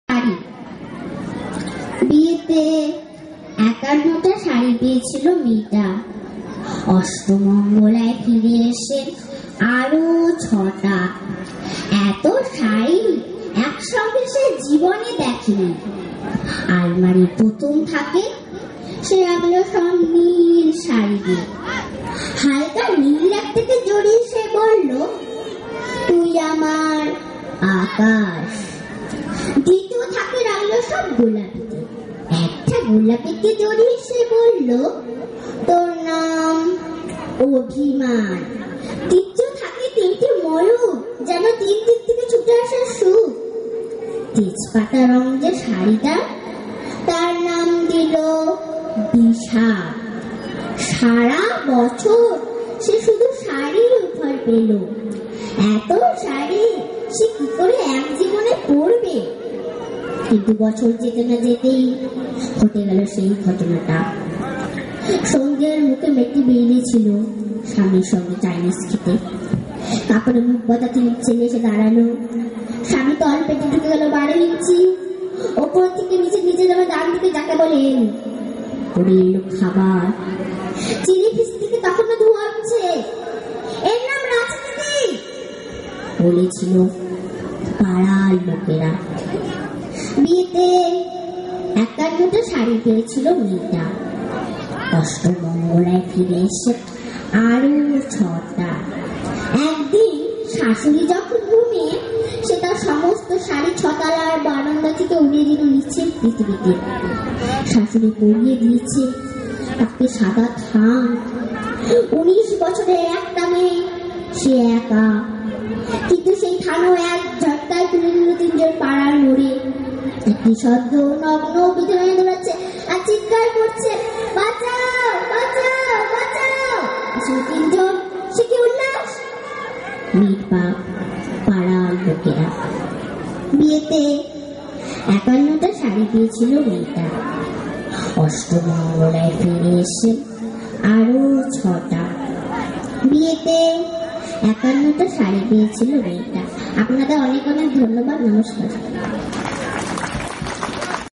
Subodh Sarkar "Shari Kabita" Abriti by NIMA Sen
Annual Cultural Programme 2016 of Manshwee Shishu Niketan
Subodh Sarkar "Shari Kabita" Abriti by NIMA Sen_My little Daghter